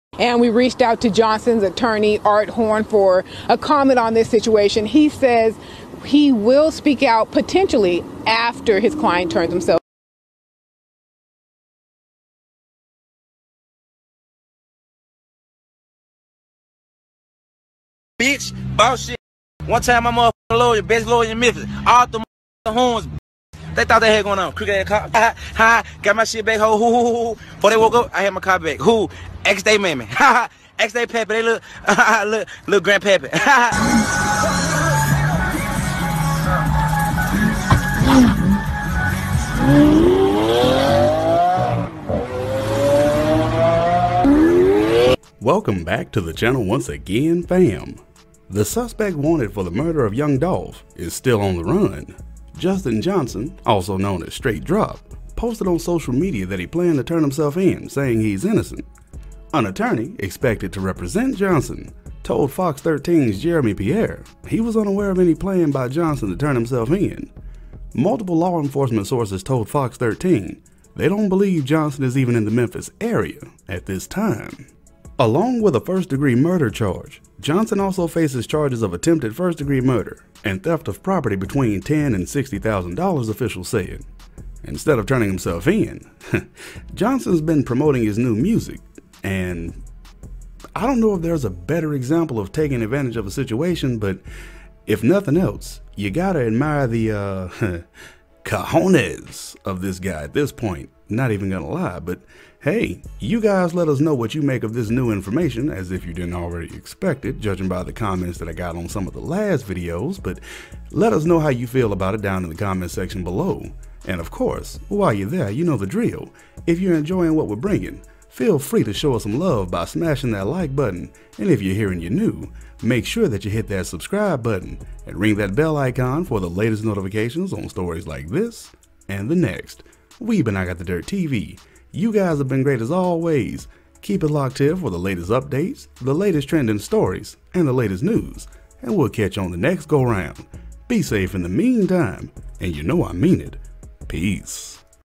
*Young Dolph Suspect Hires Yo Gotti Lawyer. Man Who Got Blac Youngsta Charges Dropped
*Young Dolph Suspect Hires Yo Gotti Lawyer. Man Who Got Blac Youngsta Charges Dropped youngdolph blacyoungsta yogotti
CORRECTION